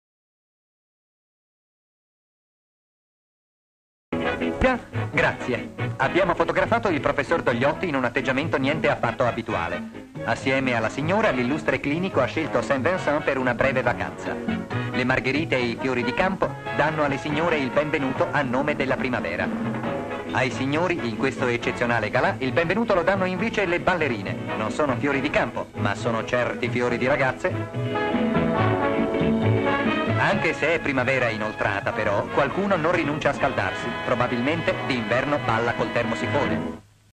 Saint Vincent festeggia, con una serata di gala, l'arrivo ufficiale della primavera"
Descrizione sequenze:un fotografo scatta una fotografia ; Dogliotti seduto ad un tavolino assapora un gelato ; una ragazza coperta da mantello e piume passa tra i tavoli offrendo i fiori ; una ragazza annusa i fiori, guarda nella cinepresa sgranando gli occhi ; una signora sorride ; le ballerine danzano sul palco ; un ballerino dal costume orientale danza agitando due fiaccole ;